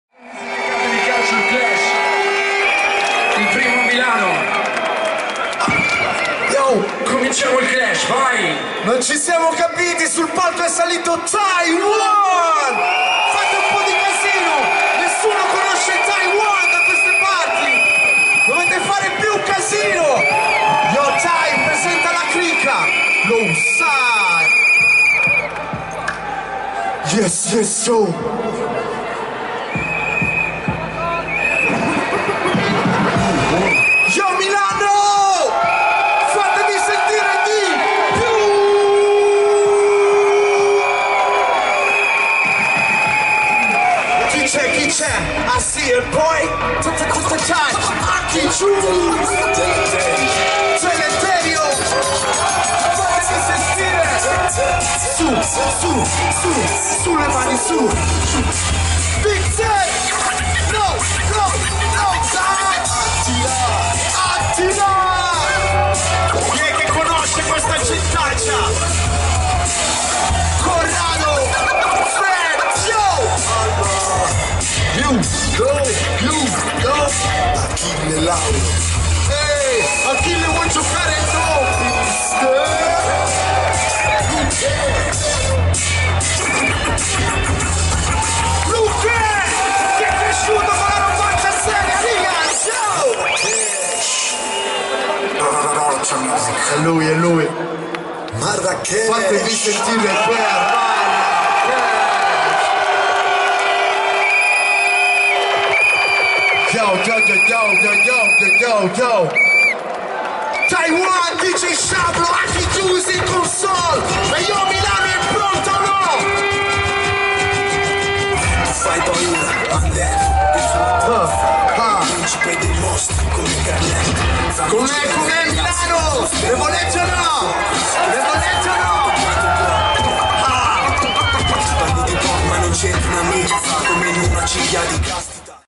Red Bull Culture Clash - Milano - Roccia Music (1° Round)
Il Red Bull Music Academy Culture Clash si è svolto per la prima volta in Italia il 24 Maggio 2014 presso la Tensostruttura Città di Lombardia (Milano). Ad aprire la serata è una delle quattro crew che si sono date battaglia a suon di musica, ovvero Roccia Music.